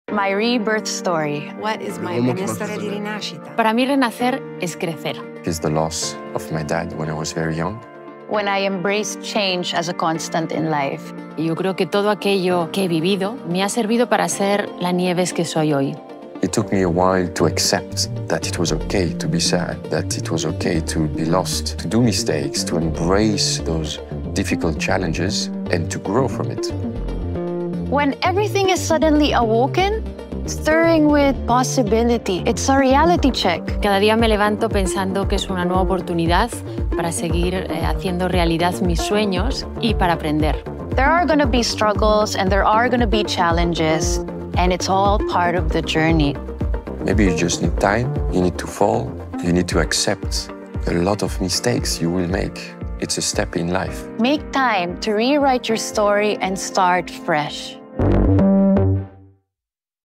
Reborn Stories: Lorenzo Viotti, Pia Wurtzbach and Nieves Alvarez | Eternally Reborn
“There will be struggles, and there will be challenges—it’s all part of the journey.” Brand ambassadors Lorenzo Viotti, Pia Wurtzbach, and friend of the Maison Nieves Alvarez embrace the quiet power of acceptance in their unique Reborn stories. And you? What's your reborn story?

Share your story with the hashtag